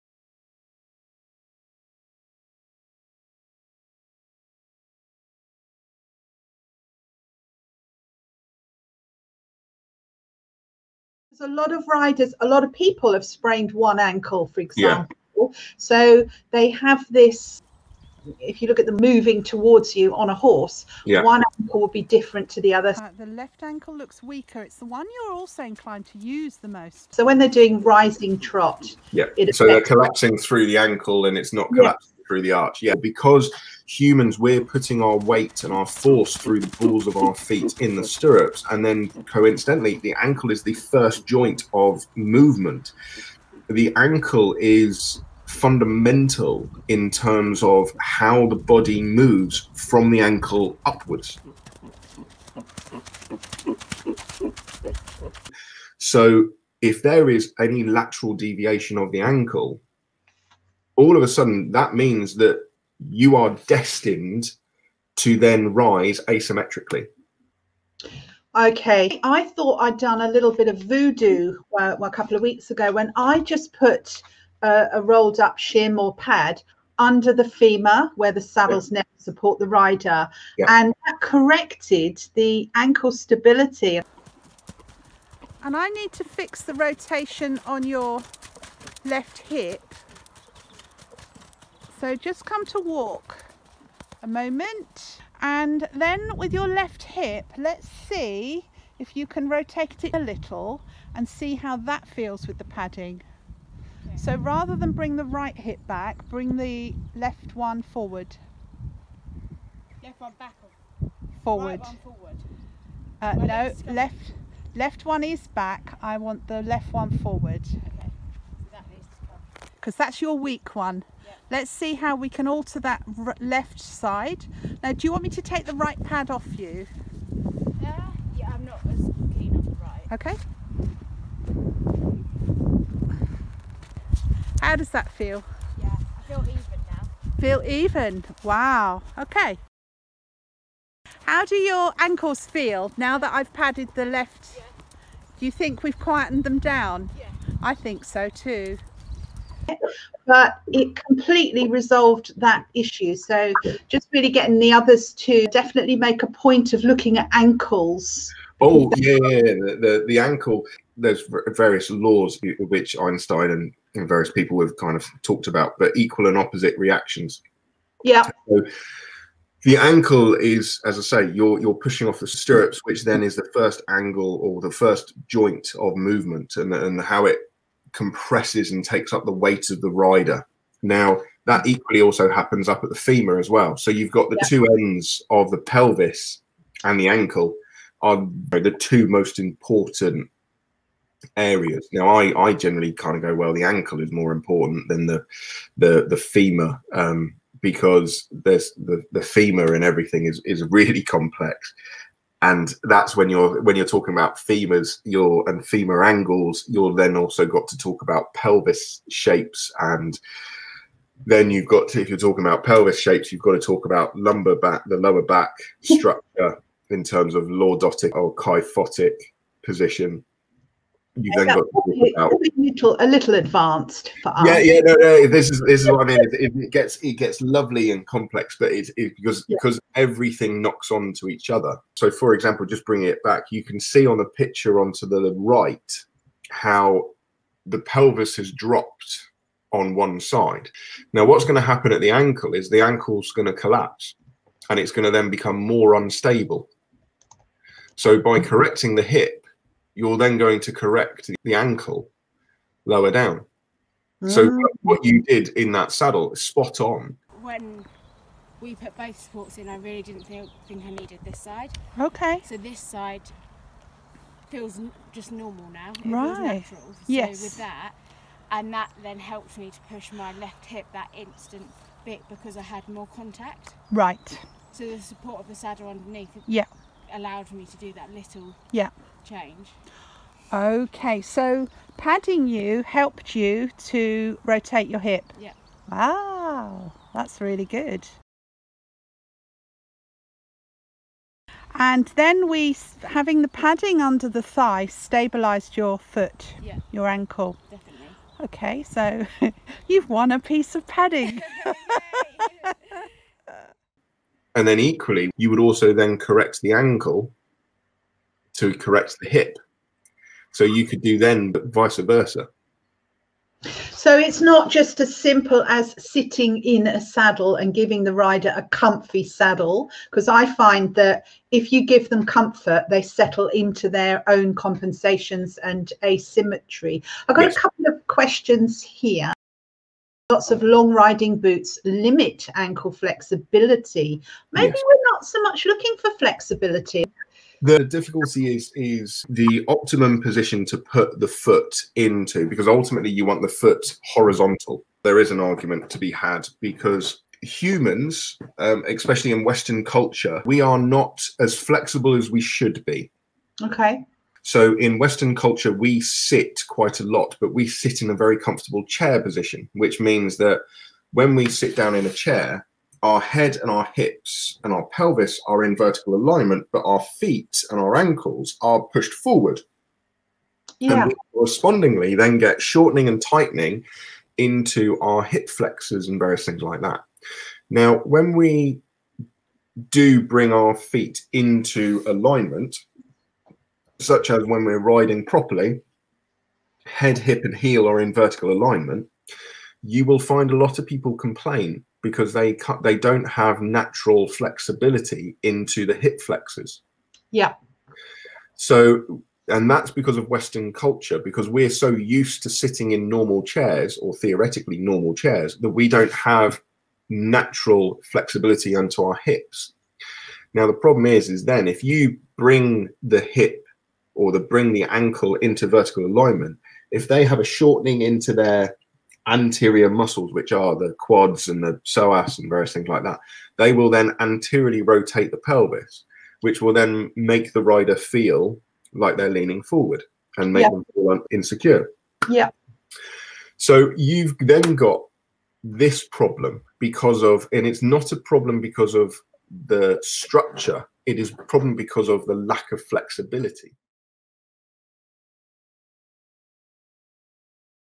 Rider Ankle Joint Functional Asymmetry and its Effects on Posture. Bradley Whale. Caroline Lindsay.
Did you know how important a role the ankles play in equitation?  Did you know how we as riders form a habitual chair seat away from the saddle?  In a 2 hour online Horse Saddle Rider Lab accredited course webinar focusing on identifying asymmetry in the horse saddle rider combination, I ask Guest lecturer Bradley Whale, horse and rider osteopath and saddle design innovator, about the effects of functional asymmetry of the rider’s ankles.  Bradley gives a detailed explanation of the knock on effects of an unstable ankle on the posture, specifically the hip joint and pelvis, together with how we habitually form a posturally poor chair seat in our everyday postural positions.

Functional asymmetry in equitation matters.